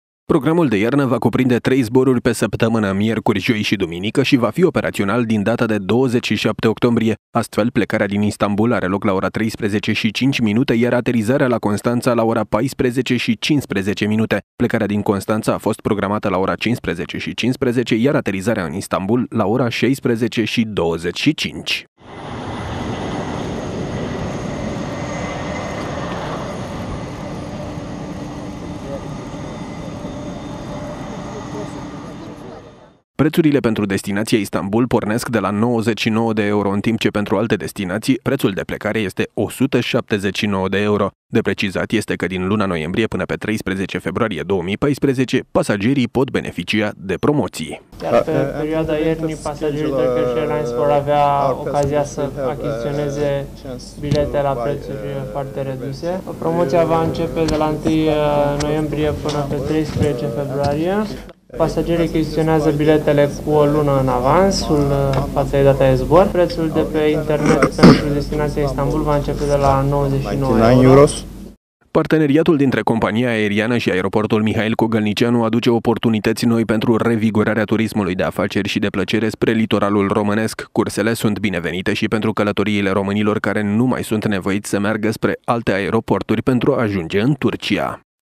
TREI ZBORURI PE SĂPTĂMÂNĂ
Turkish Airlines a organizat pe Aeroportul Mihail Kogâlniceanu o conferință de presă. Cu această ocazie s-a făcut public programul de iarnă al companiei, precum și zborurile care vor fi operate pe ruta Constanta- Turcia.